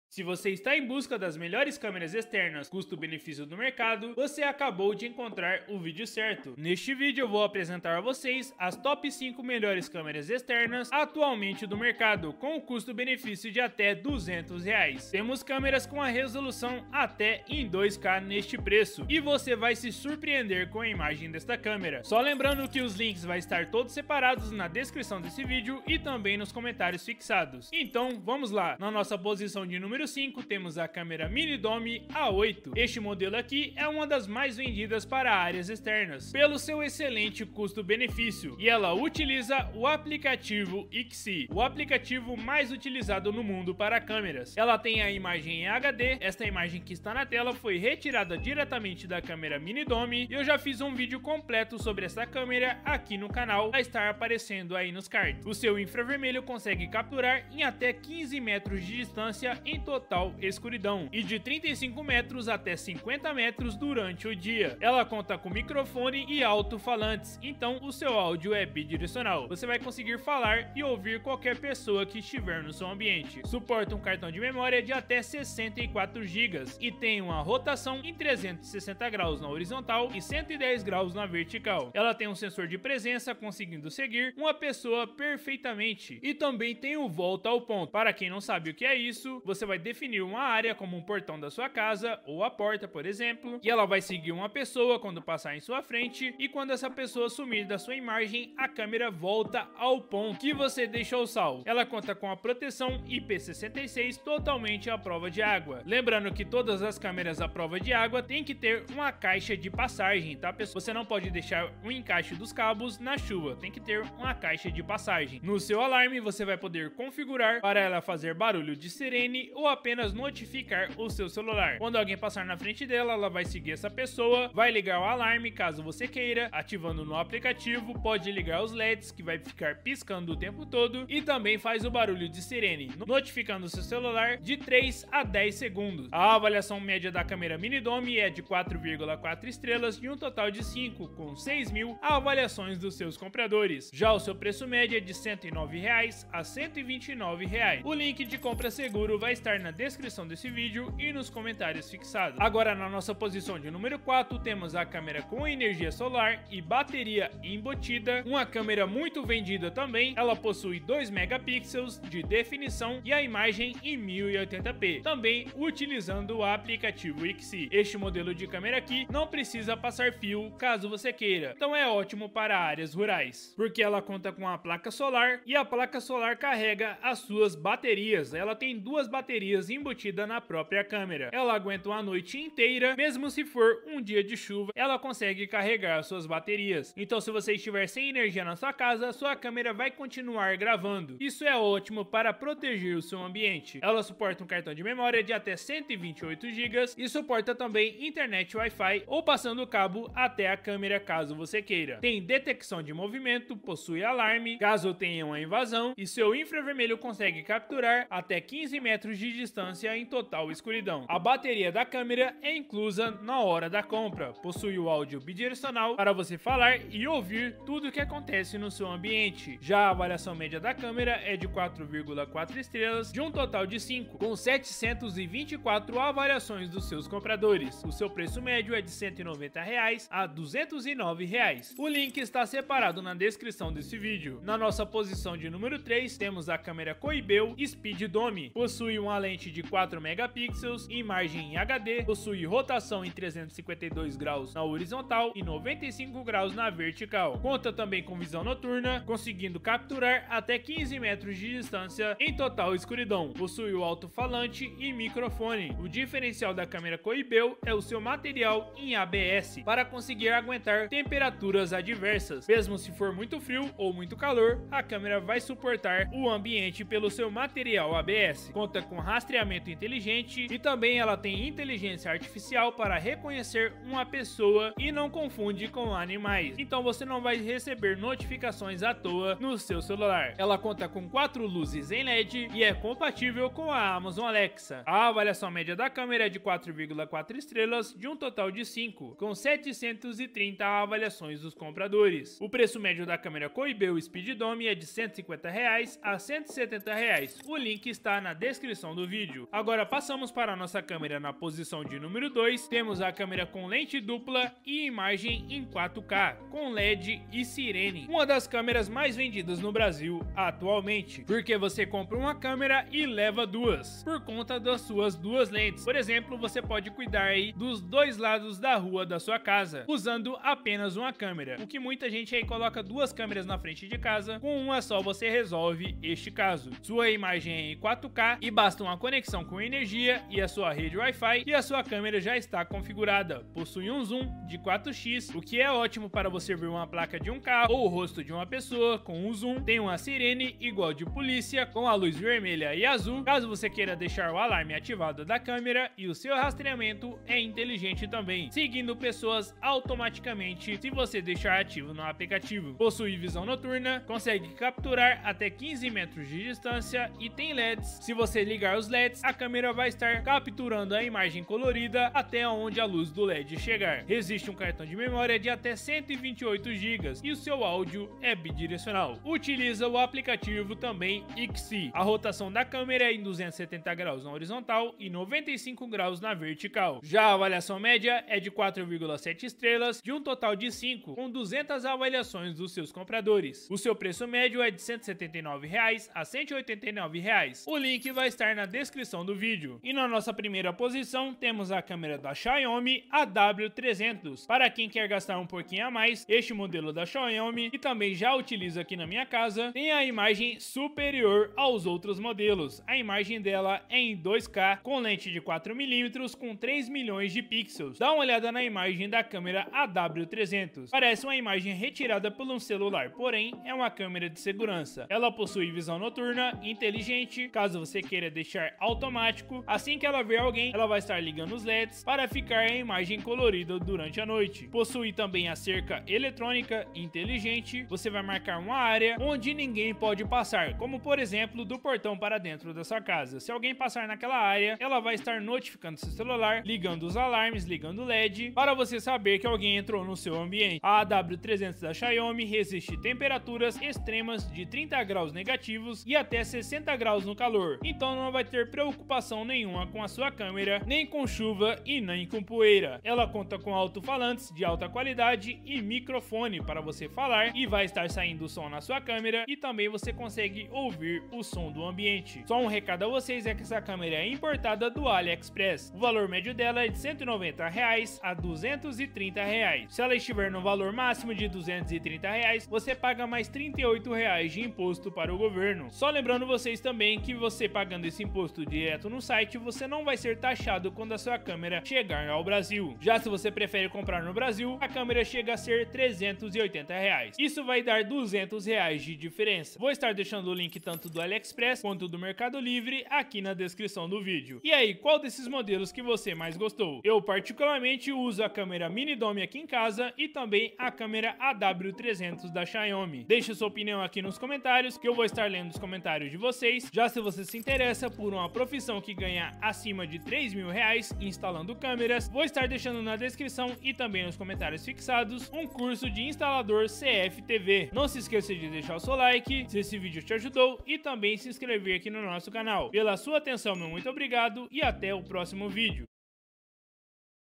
QUAL AS MELHORES CÂMERAS DE SEGURANÇA CUSTO BENEFÍCIO IP WIFI de 2025! ✅
1 - Xiaomi AW300

5 - Câmera Mini Dome

CÂMERAS QUE RECOMENDO!!!
Vale a pena investir em um cartão de memoria para durar muito tempo de gravação esses são os que funciona na câmera:
Cartão de Memoria 16 GB
Cartão de Memoria 32 GB
Cartão de Memoria 64 GB
Cartão de Memoria 128 GB

Está procurando as melhores câmeras de segurança custo-benefício para 2025? Neste vídeo, eu, Rodrigo Ferrari, apresento as top escolhas de câmeras IP WiFi que oferecem o melhor equilíbrio entre preço e qualidade. Descubra quais são as melhores opções para proteger sua casa ou negócio sem gastar muito!

📸 Temas abordados neste vídeo:

Melhores câmeras de segurança IP WiFi de 2025
Câmeras de segurança com o melhor custo-benefício
Comparação das principais câmeras externas de segurança
Dicas para escolher a melhor câmera de segurança para suas necessidades